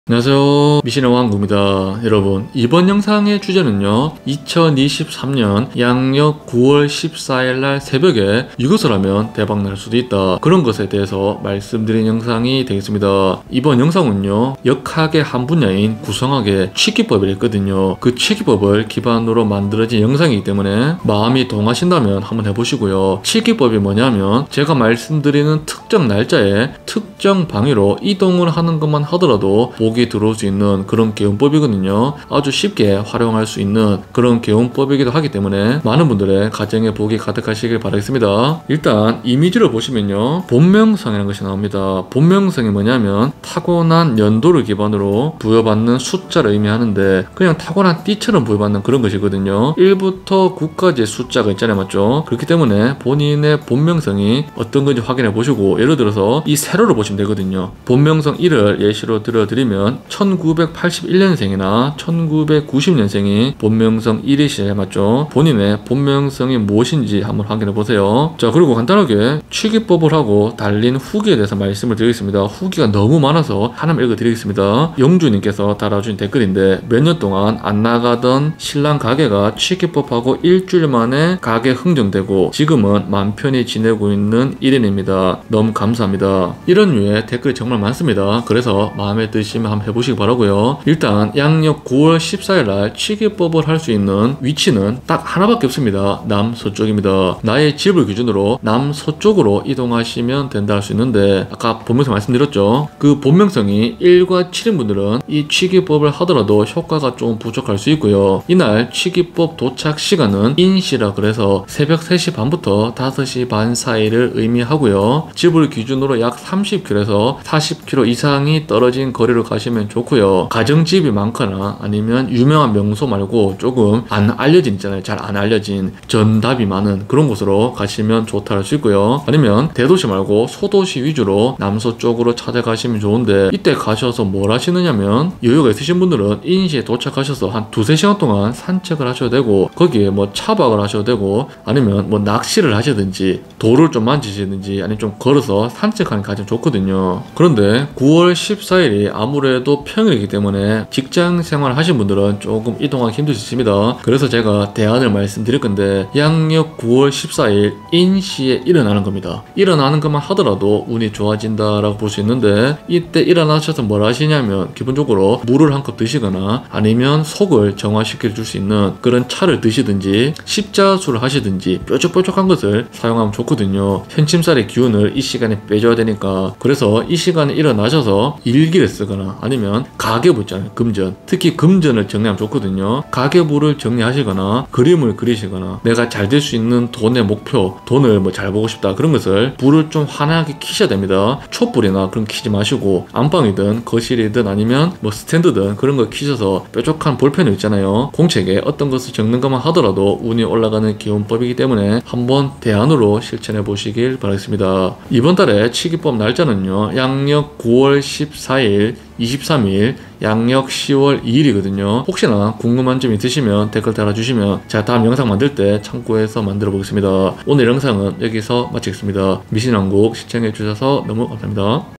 양력 9월 14일 새벽에 "이것" 해보세요 !! 큰 돈이 들어올수도 있어요 !!
이번 영상은 양력 9월 14일 취기법 관련 영상입니다
취기법을 실천하고 많은분들이 복을 받거나 대박난 경우가 많아요
모든 분들에게 좋을순 없지만 마음이 동하신다면
한번 실천해보시길 바랍니다